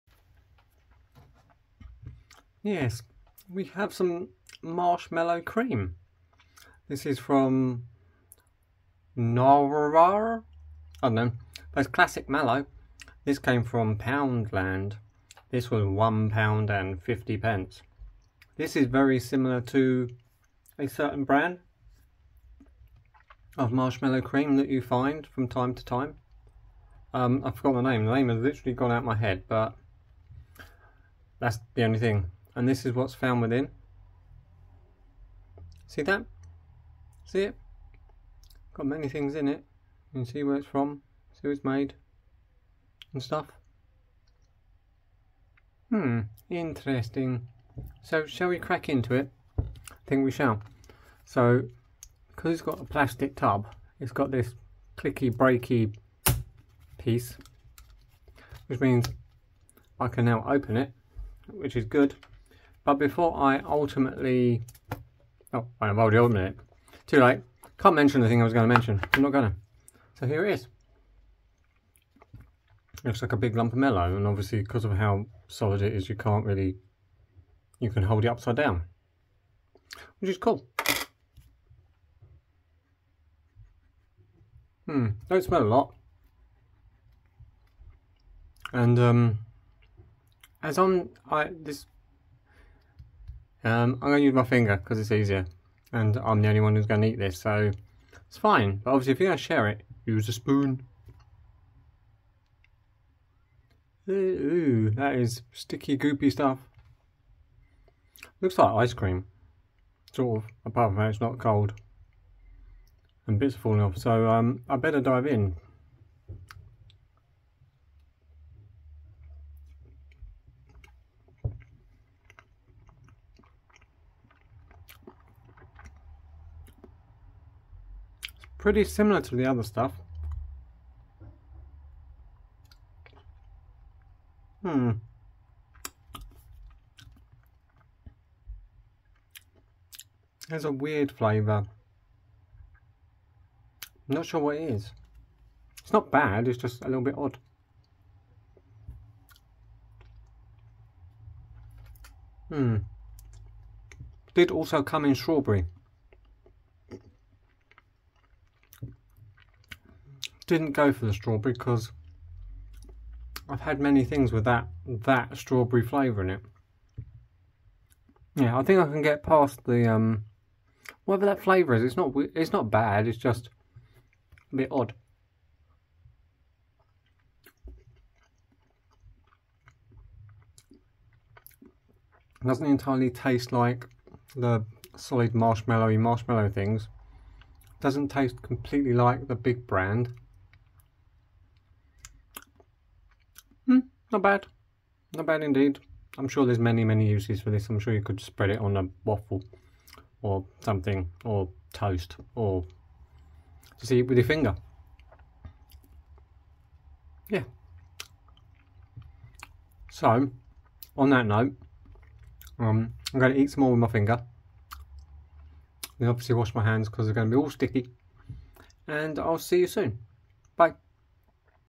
Navarre Classic Mallow Marshmallow Cream - Random Reviews
How will this Marshmallow Cream stack up to the Fluff.